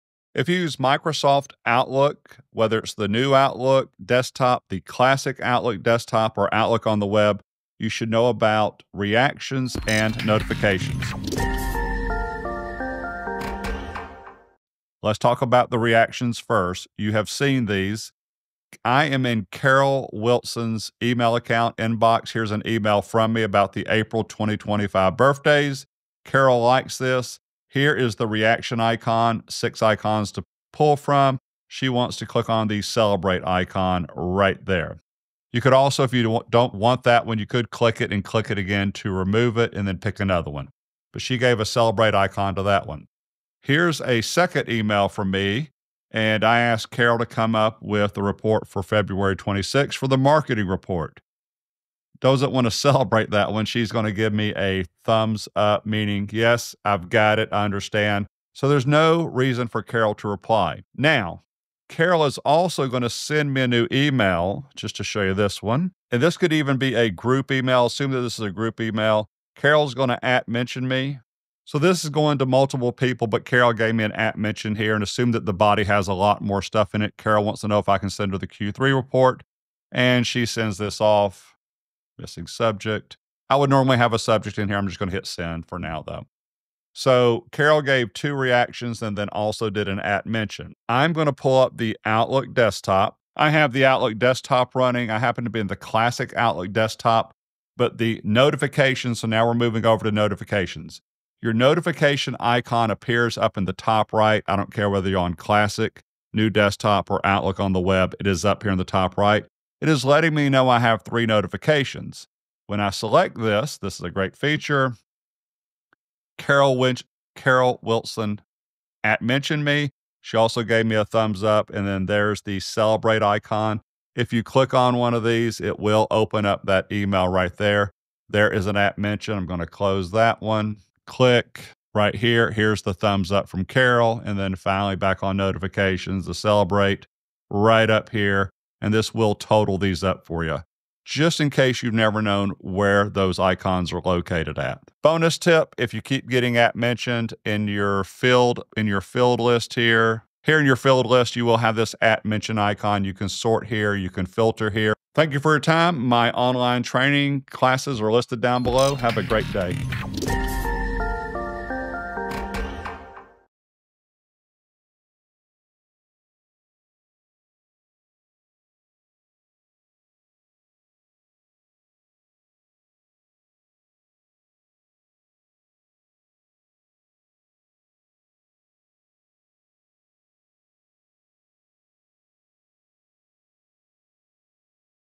Outlook: Reactions & Notifications & At Mentions
With Outlook, you should leverage reactions, notifications, and mentions. Whether using the new or classic desktop versions or Outlook on the web. Learn how to react to emails with various icons and manage your notifications easily. I'll show you the steps using examples like @ mentioning in group emails and navigating the notification icon in the top right corner to keep track of your mentions and reactions efficiently.

Chapters/Bookmarks
00:00 Introduction to Outlook Reactions and Notifications
00:14 Exploring Reactions in Outlook
00:18 Using Reactions in Emails
01:00 Sending Emails with @ Mentions
01:39 Introduction to Notifications
01:41 Navigating Notification Icons
02:24 Bonus Tips and ConclusionAnd make sure you subscribe to my channel!

✅Understanding Reactions in Outlook
If you're using Microsoft Outlook, whether it's the new Outlook desktop, the classic Outlook desktop, or Outlook on the web, it's crucial to leverage the reactions feature. Reactions allow users to express themselves quickly and efficiently without the need to draft a lengthy response.

Let's dive into how reactions work using a practical example. Imagine you're in Carol Wilson's email account inbox. Carol receives an email about upcoming birthdays and decides to show her enthusiasm by clicking on the 'celebrate' icon. With a simple click, her reaction is sent, reducing the need for an additional email.

Reactions offer six different icons to choose from, allowing users to express a range of emotions or acknowledgments. For instance, when Carol receives a request to prepare a report, she opts for a 'thumbs up' reaction, indicating her acknowledgment and understanding without sending a separate email reply.

✅Notifications: Stay Updated Effortlessly
Notifications in Outlook are your digital assistant, ensuring you don't miss out on any crucial updates. Your notification icon, conveniently located at the top right corner, alerts you to any new interactions or mentions.

Whether you're using the classic Outlook desktop or the web version, this feature remains consistent. Clicking on the notification icon reveals a list of updates, such as reactions or mentions from colleagues, helping you stay informed and responsive.

For example, when Carol mentions you in an email, you receive a notification. This feature ensures you remain in the loop without sifting through numerous emails, streamlining your workflow.

✅At Mentions: Direct Attention Where It's Needed
At mentions are a powerful tool in Outlook, allowing users to direct attention to specific individuals in group emails. This feature ensures that important requests or information don't get lost in the shuffle of group communication.

Consider a scenario where Carol sends a group email with specific tasks for different team members. By using at mentions, she can highlight who needs to focus on what, ensuring clarity and prompt responses.

This feature is particularly useful in large teams where individual responsibilities need to be clearly defined. It helps streamline communication, making it more efficient and effective.

-- EQUIPMENT USED
○ My camera